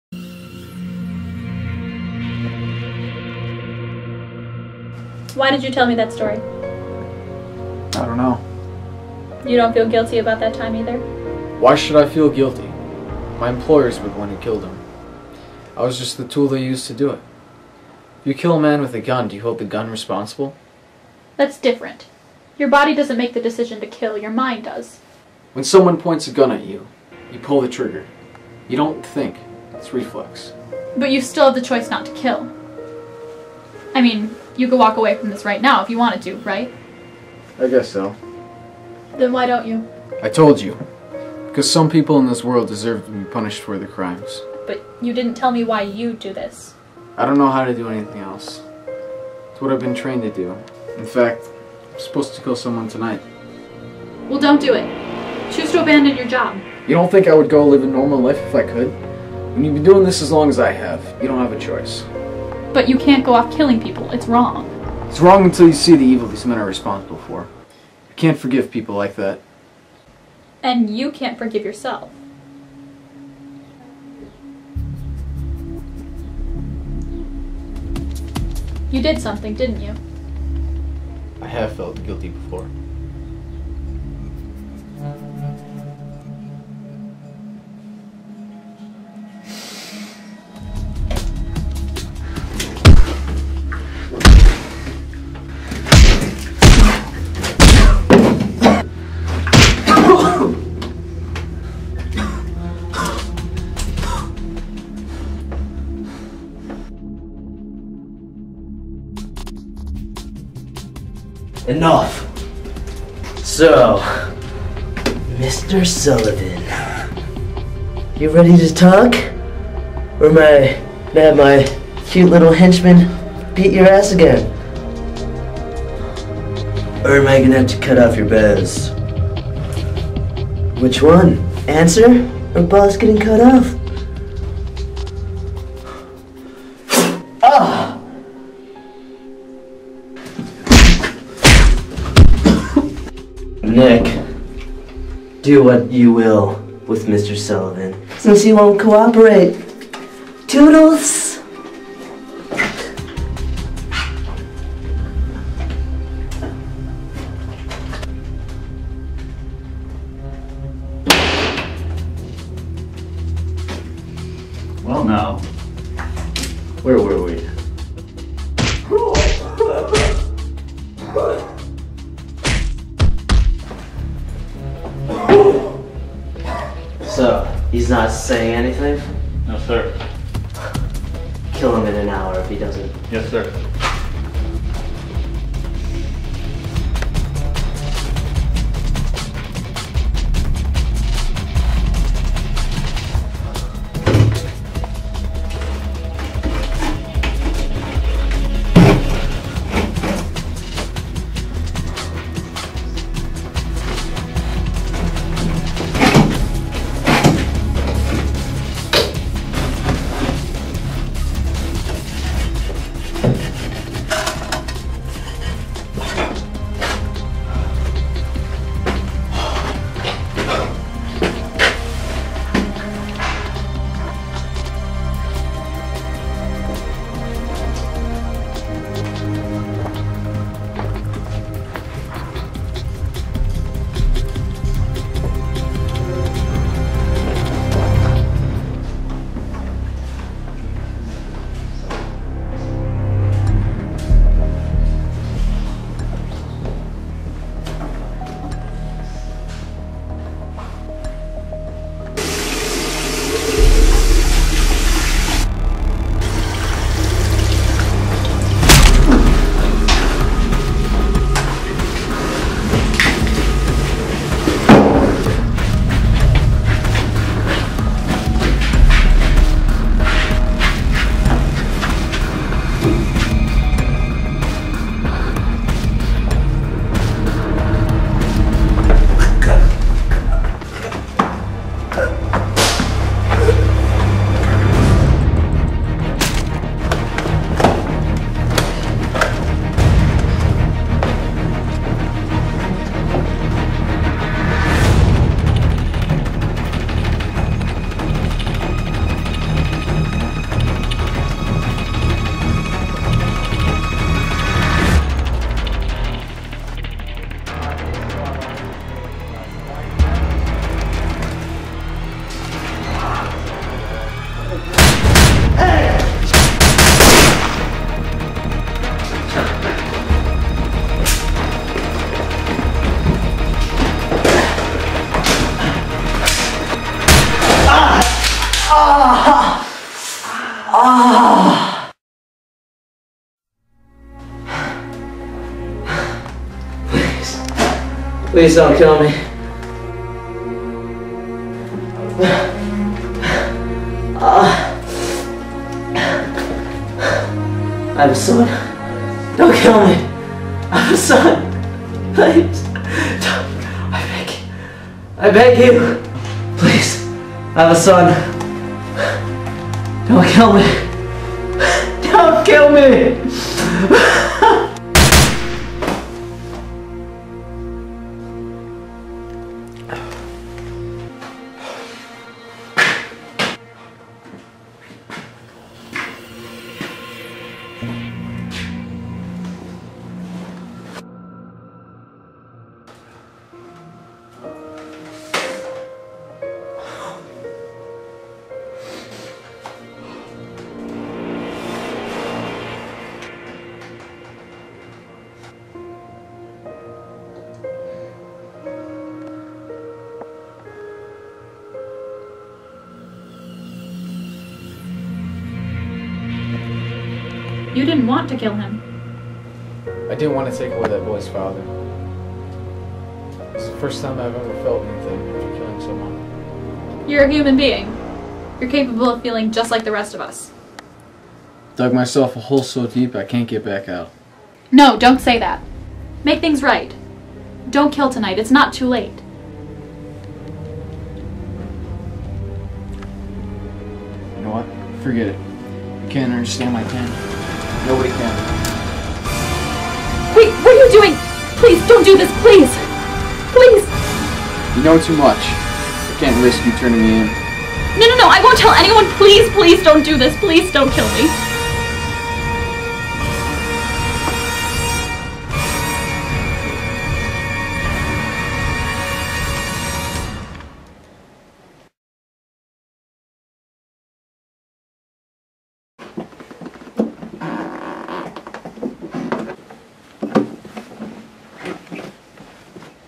Redemption: Part Three
Film Lit movie for Grosse Pointe South High School. A hitman is seeking absolution for his violent past and visits a psychiatrist in desperation and need of help.